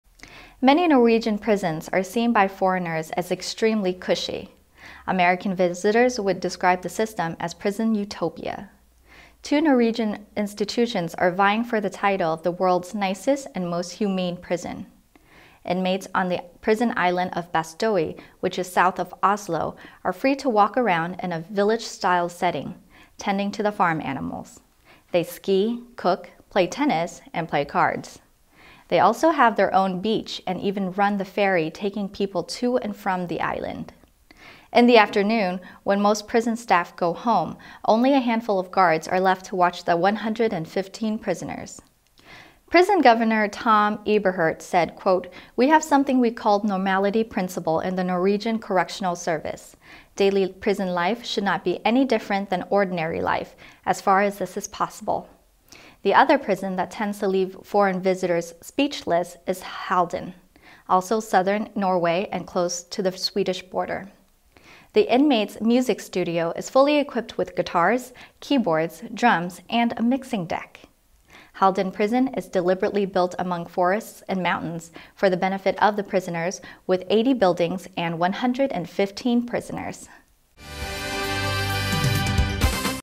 Many Norwegian prison systems described as "prison utopia".
Many Norwegian prisons are seen by foreigners as extremely cushy.  American visitors would describe the system as "prison utopia".Two Norwegian institutions are vying for the title of the world's "nicest" or "most humane" prison.Inmates on the prison island of Bastoey, which is south of Oslo, are free to walk around in a village-style setting, tending to farm animals.